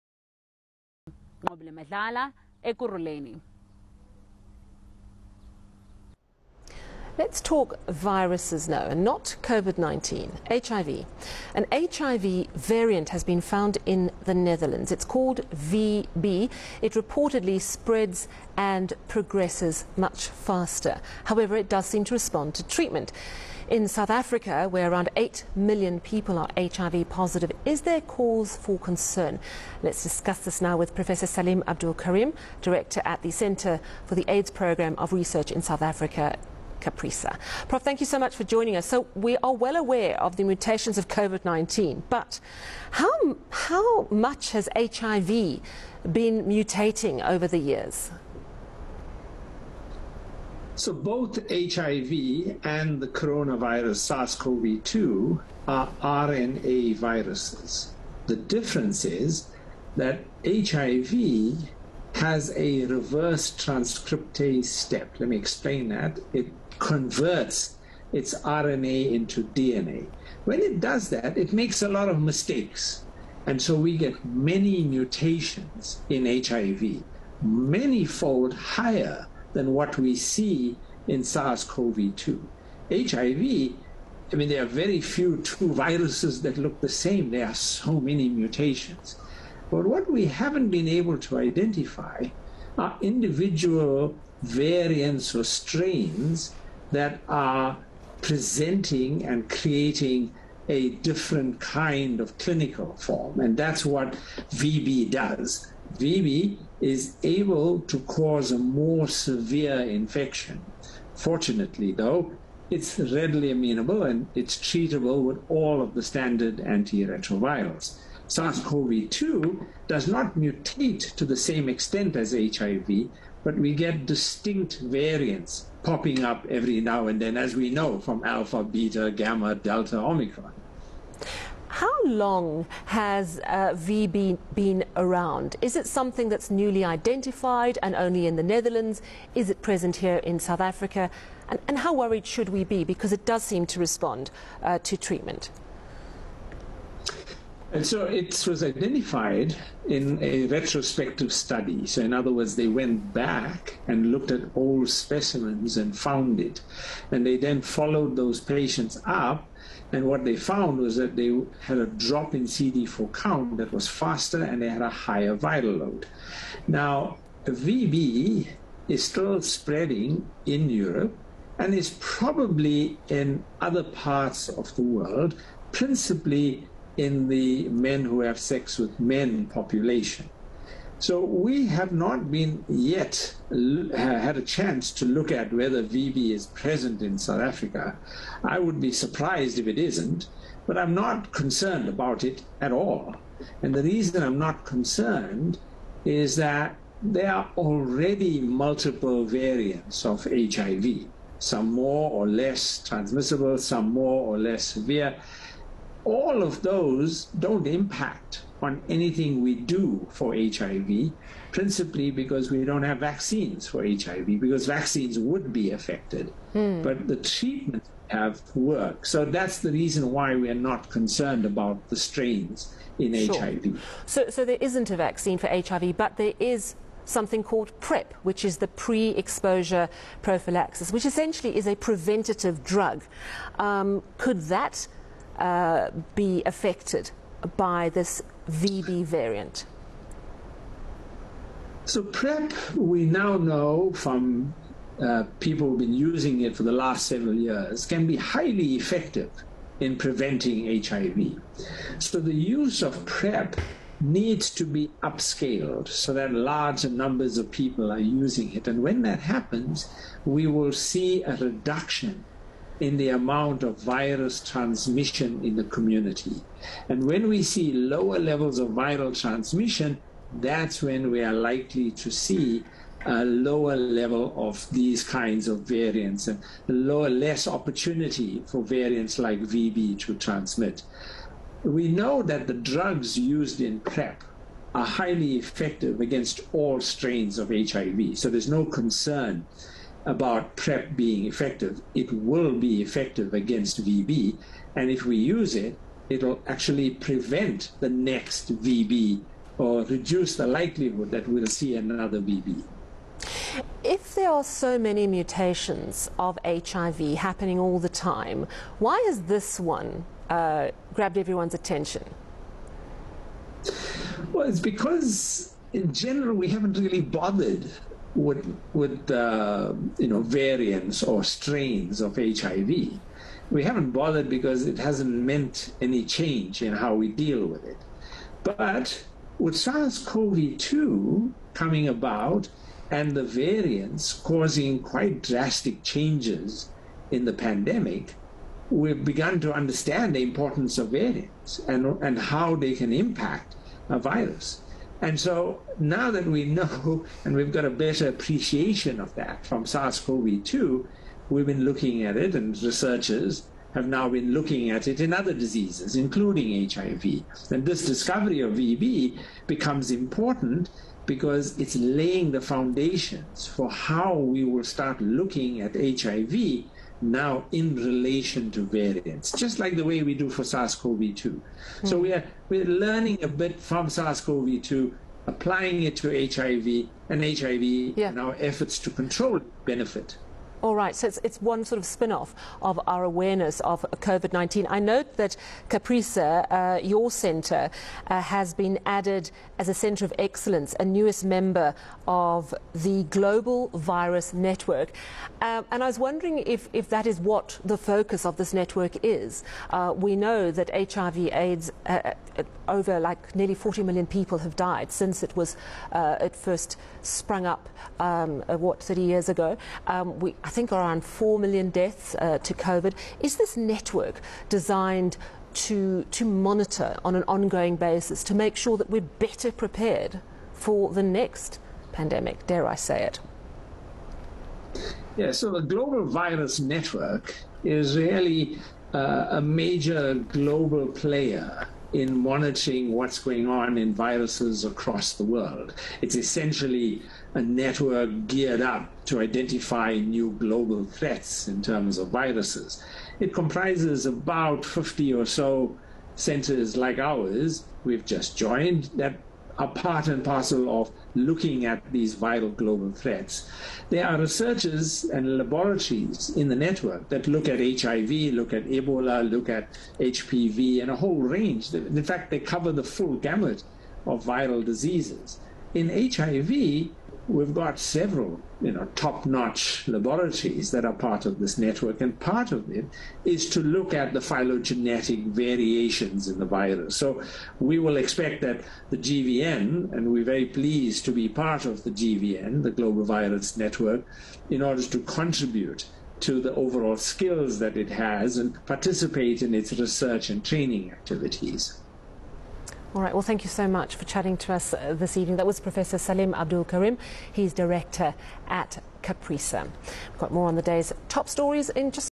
New HIV Variant found in the Netherlands
An HIV variant called VB has been found in the Netherlands. It reportedly spreads and progresses faster. In a country such as South Africa, where around eight million people are HIV positive, is there cause for concern? Professor Salim Abdool Karim, weighs in